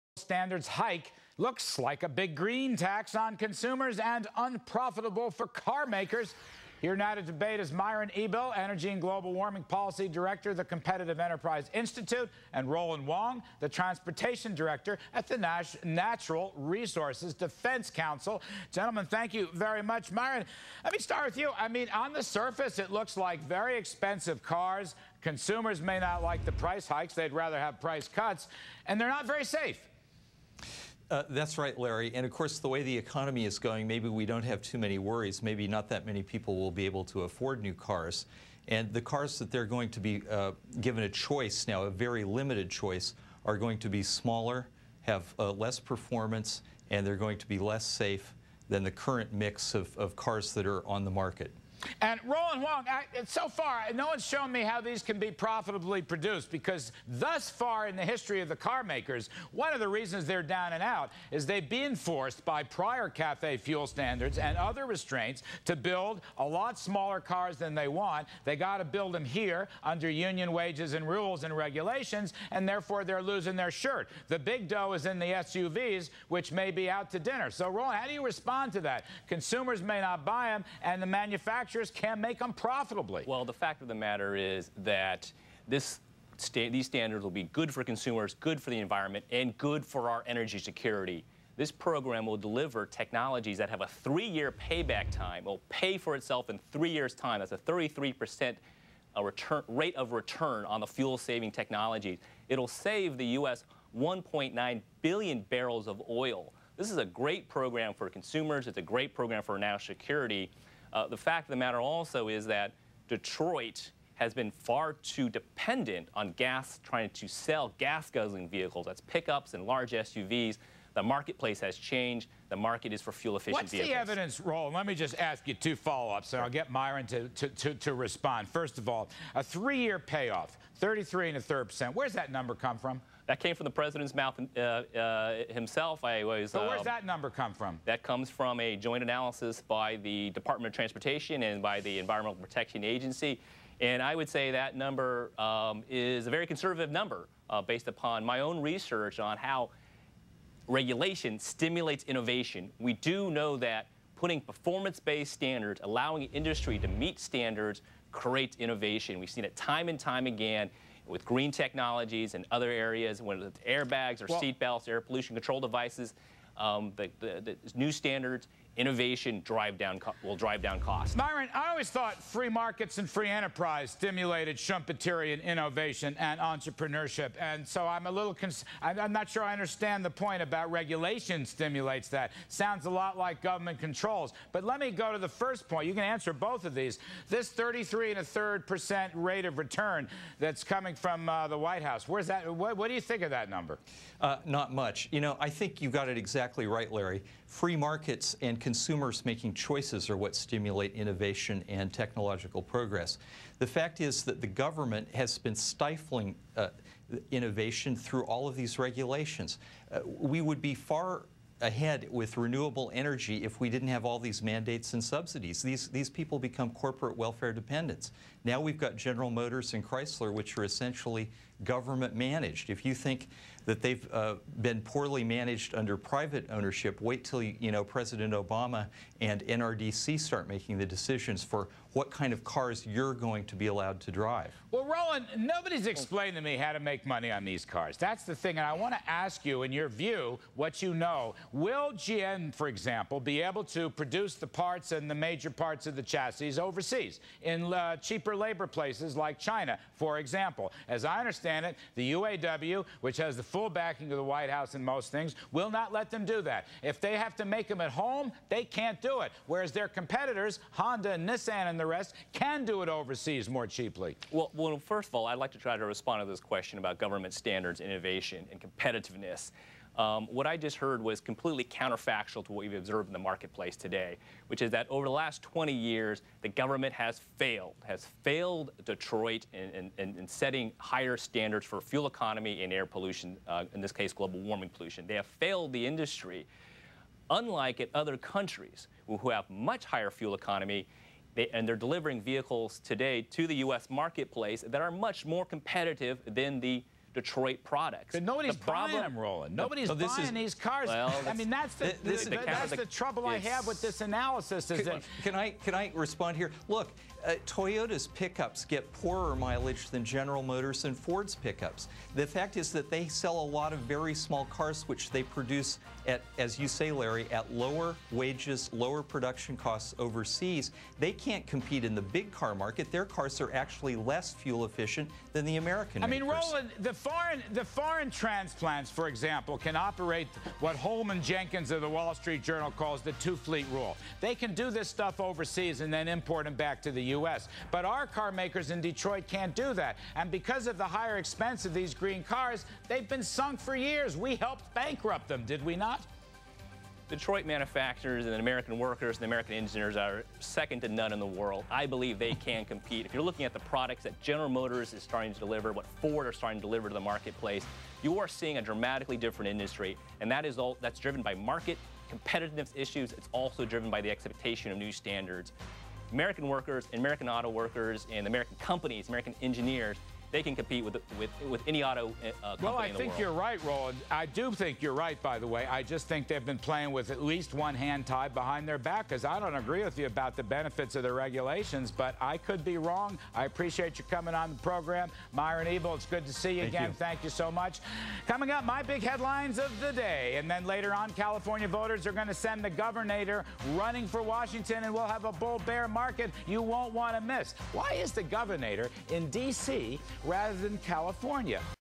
Myron Ebell on Car Mileage Regulations
Competitive Enterprise Institute Director of Energy Policy Myron Ebell debates the White House's proposal to raise miles per gallon regulations on car manufacturers.